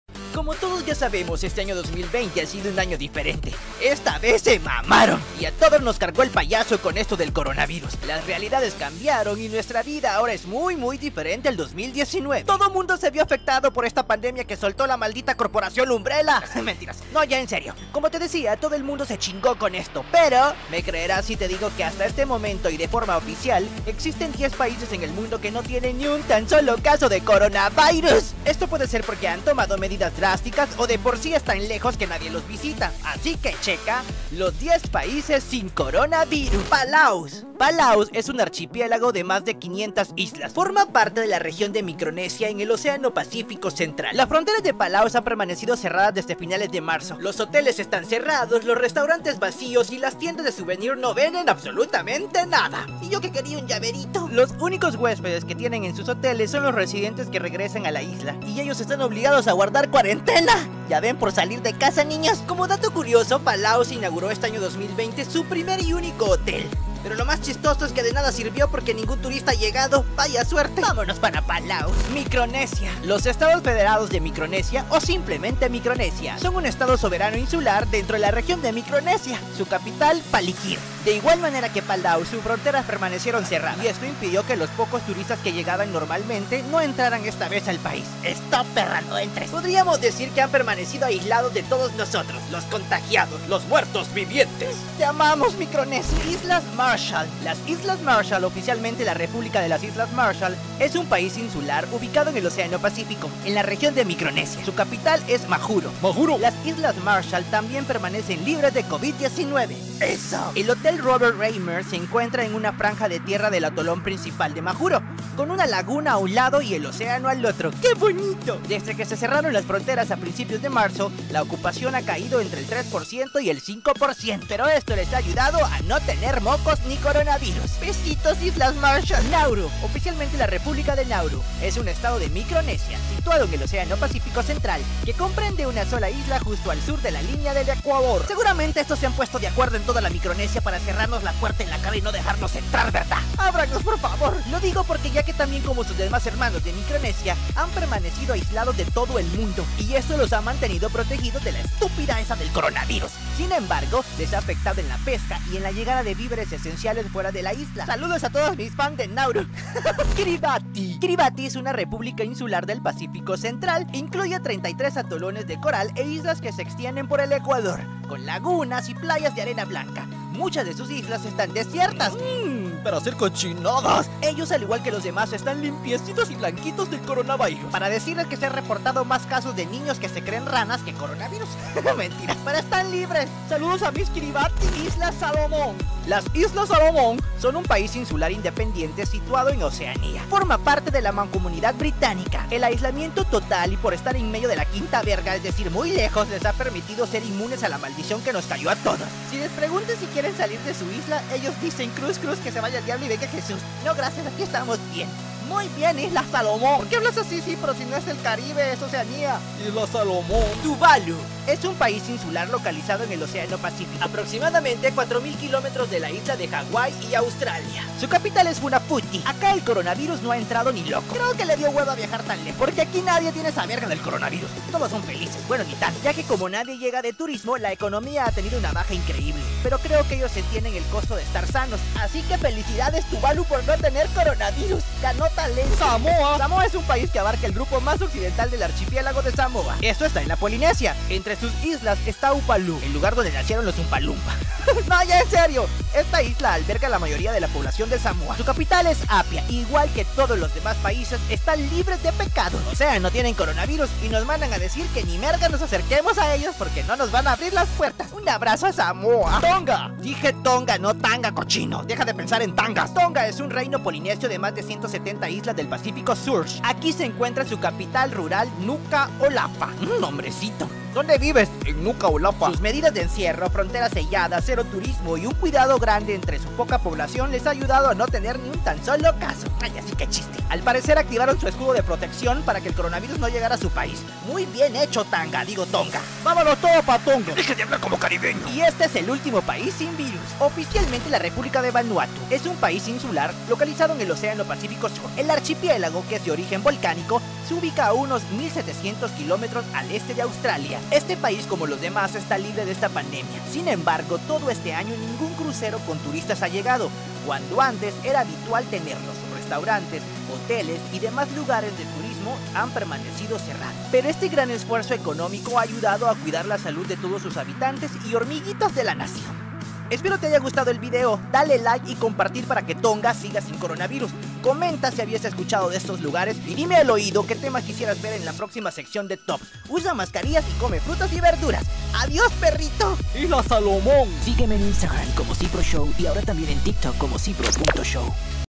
ESTOS SON LOS ÚNICOS PAÍSES SIN COVID-19
Aquí no ha llegado el Coronavirus, será que ganan la batalla o también llegará la pandemia a ellos.